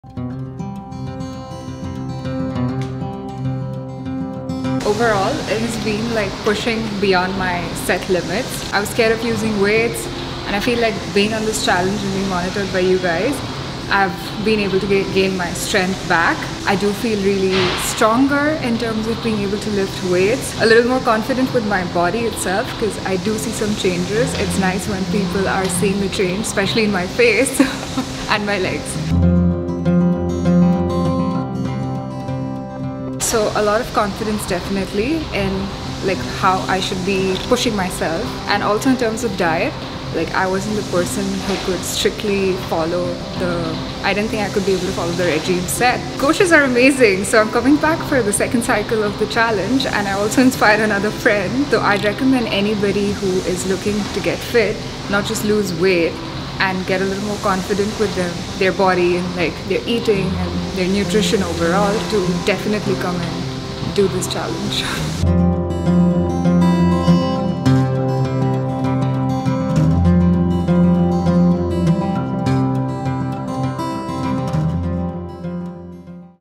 Hear from Pallavi, one of our @bestbodyco 6in4challenge members
Hear from Pallavi, one of our @bestbodyco 6in4challenge members who has completed multiple months of our program!

"It has pushed me beyond my limits. I used to be scared of using weights, but on this challenge, it's monitored really well and I've gained more confidence and strength. I see changes in my body. It's nice when people see changes in my face and my legs!

The coaches are amazing, I've gotten my friends to join too. I recommend anybody who is looking to get fit, not just lose weight, but to get more confident with their eating and nutrition, then definitely come and do this challenge!"